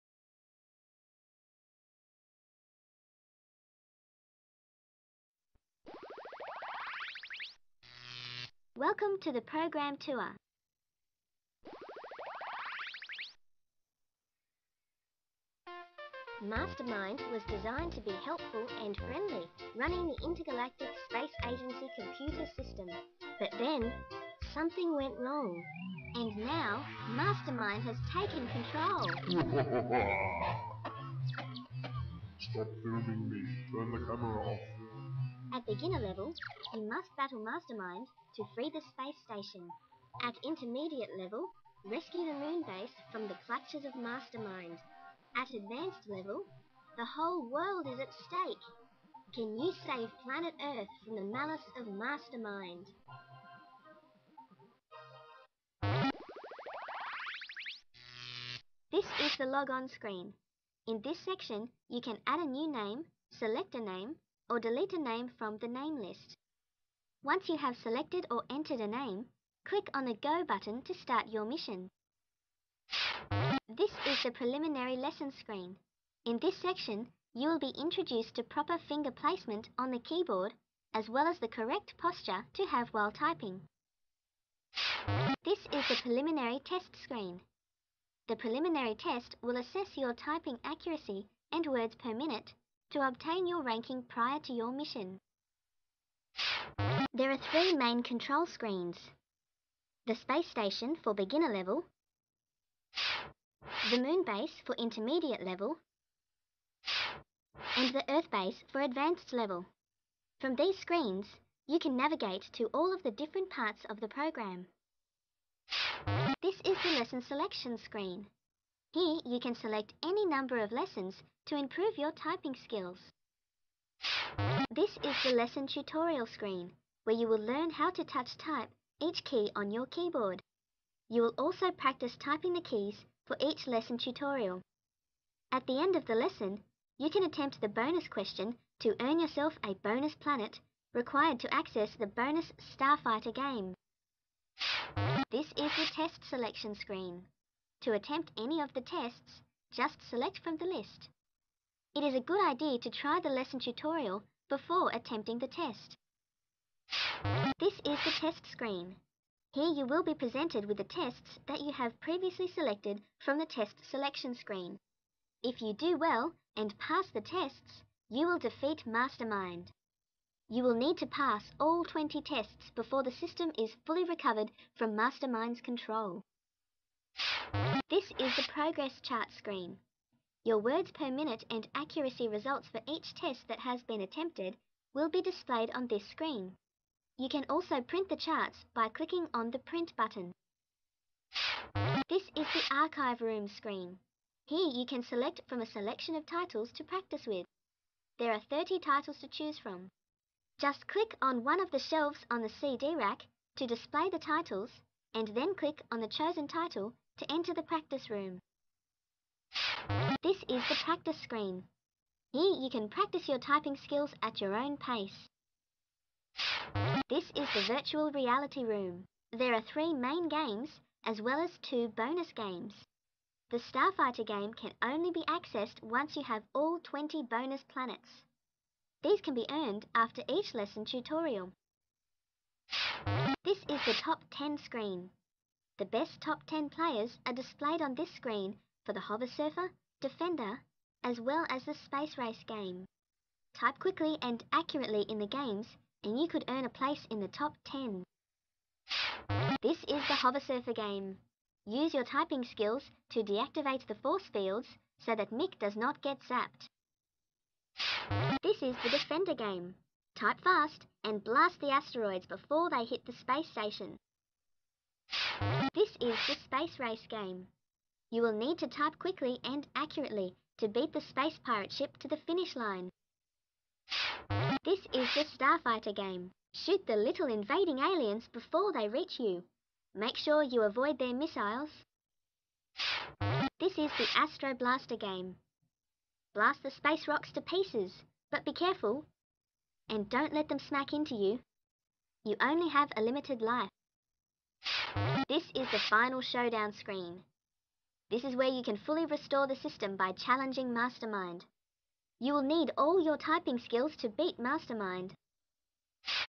Eureka's Kids Typing Tutor
This is a program tour of Eureka's Kids Typing Tutor program, which is now available on the Mac App Store.